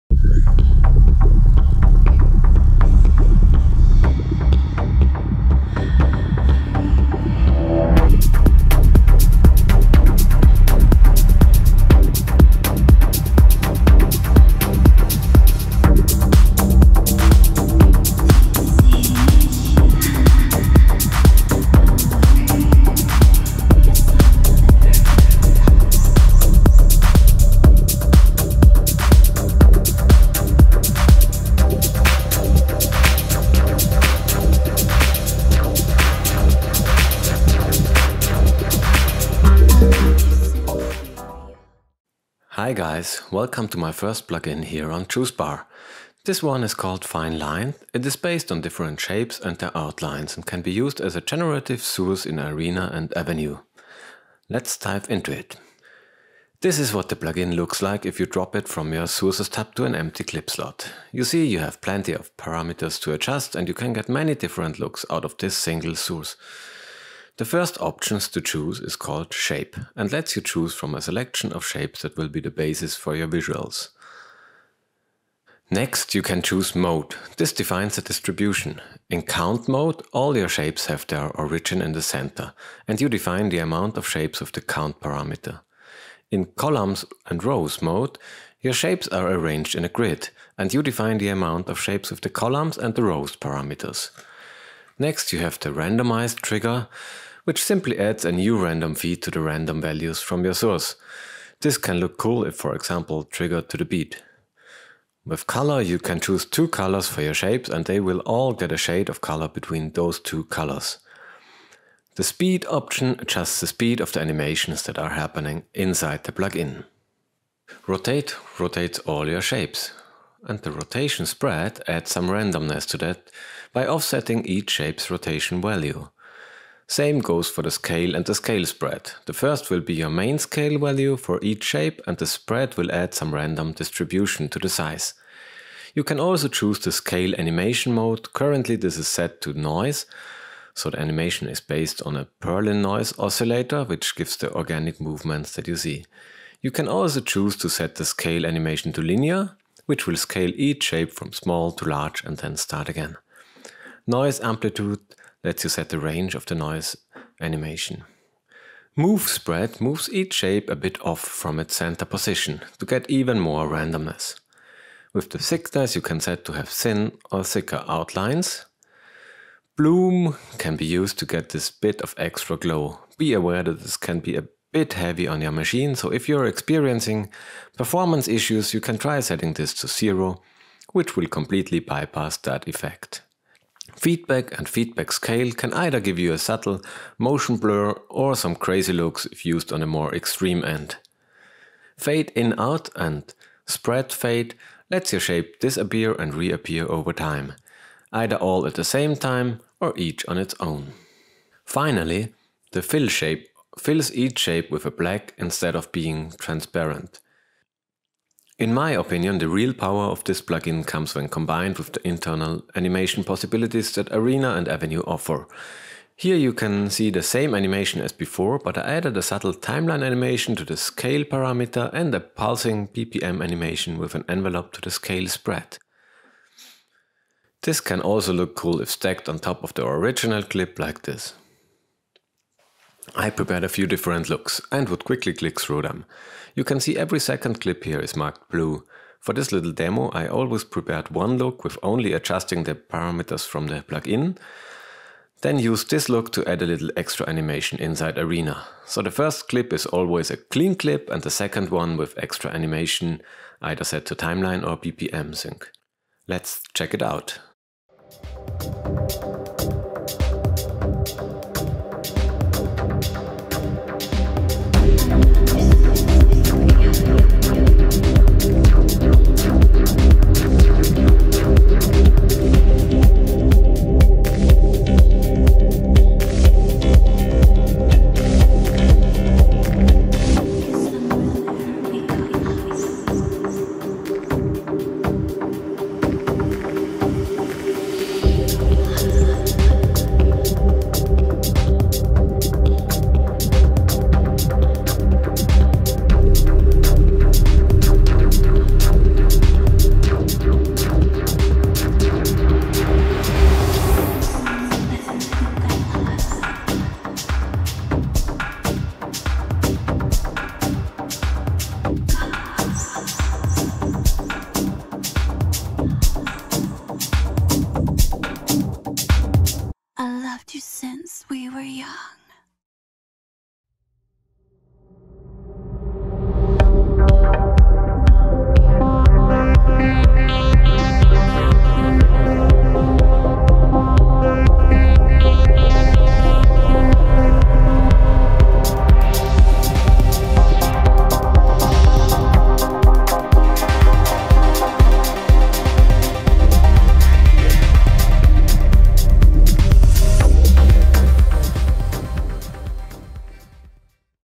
Resolume Plugin: FineLined
This video show you how to use the FineLined plugin in Resolume with JuiceBar.